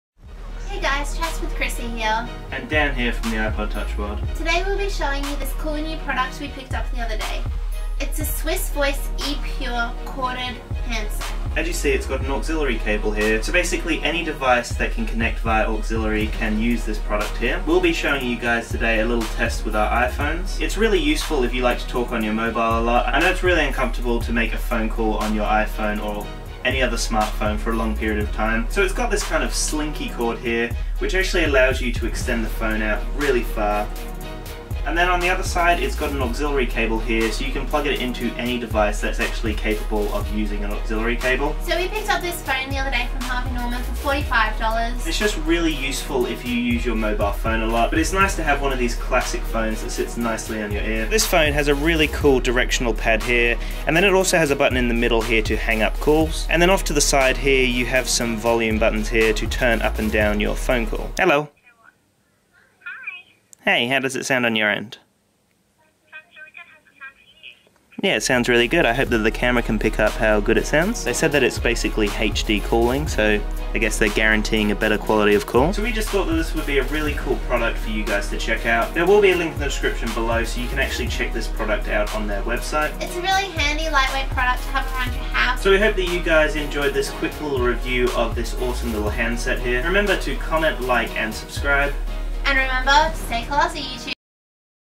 ePure SwissVoice Corded Handset Review Ft. TheIpodTouchWorld
In this video Dan from TheIpodTouchWorld and I take a closer look at the ePure SwissVoice Corded Handset and give you our thoughts on it!!